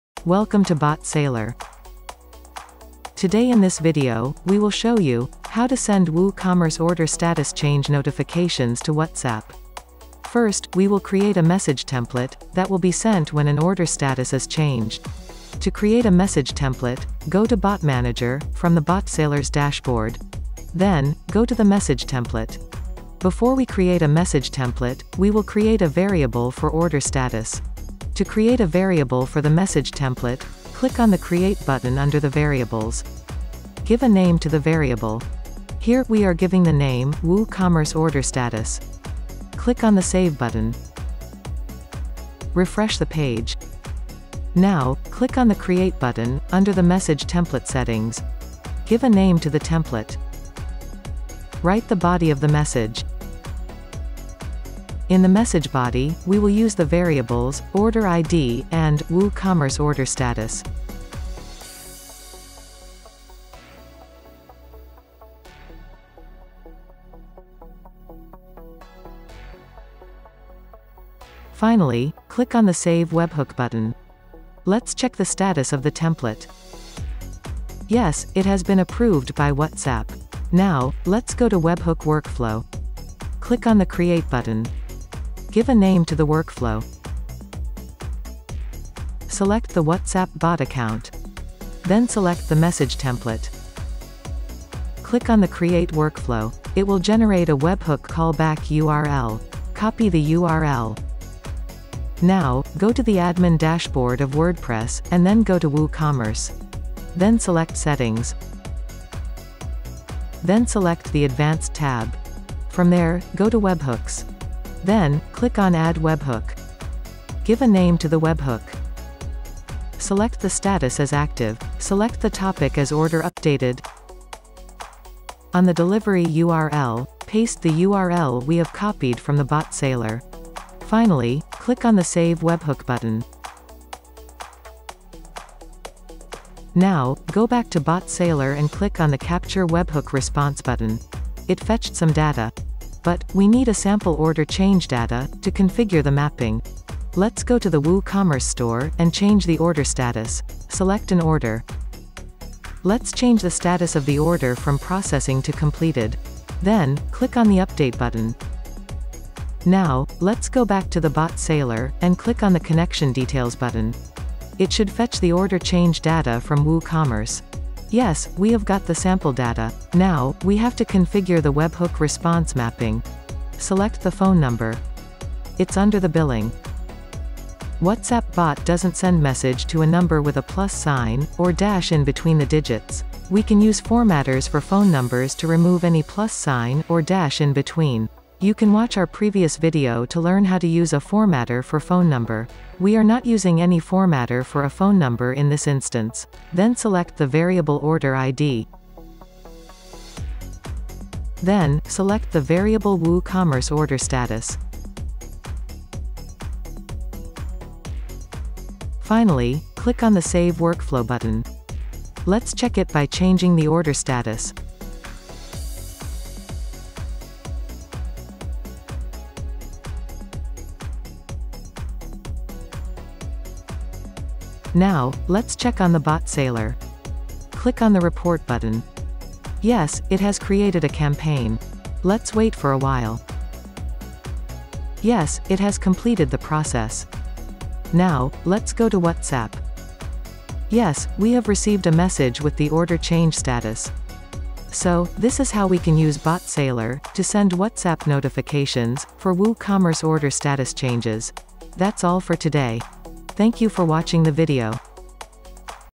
How to Send WooCommerce Order Status Change Notification to WhatsApp Webhook Workflow
In this video, we have shown how to send WooCommerce order status change notifications to WhatsApp.

Now, you can send a status change notification to customers WhatsApp by using BotSailor.

Get free access to the BotSailor and enjoy free Telegram and WhatsApp Bot marketing for your business: